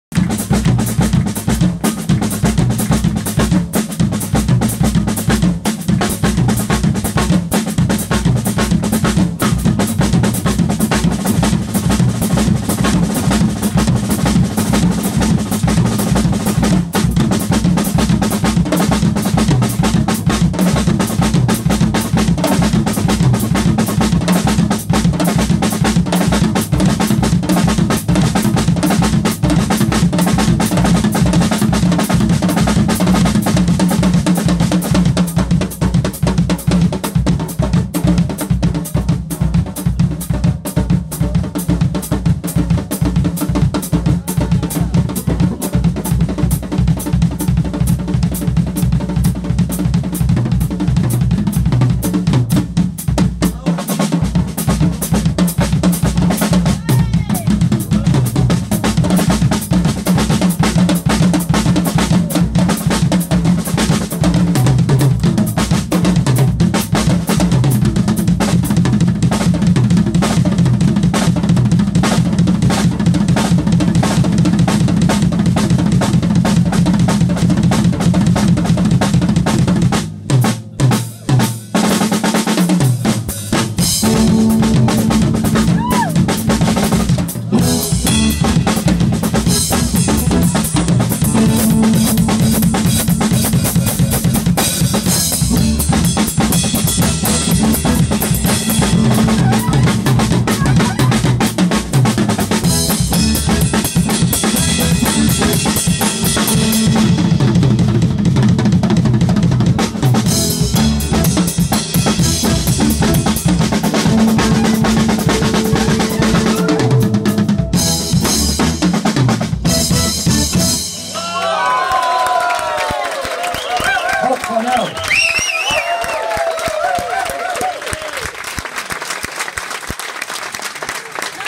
Peter White at PizzaExpress Jazz Club - Marc Parnell Drum Solo - Holborn - London - 3 Nove 2017 - 2
Peter White (guitar)
Oli Silk (keyboard)
Carl Stanley (sax)
Frank Felix (bass)
Marc Parnell (drums)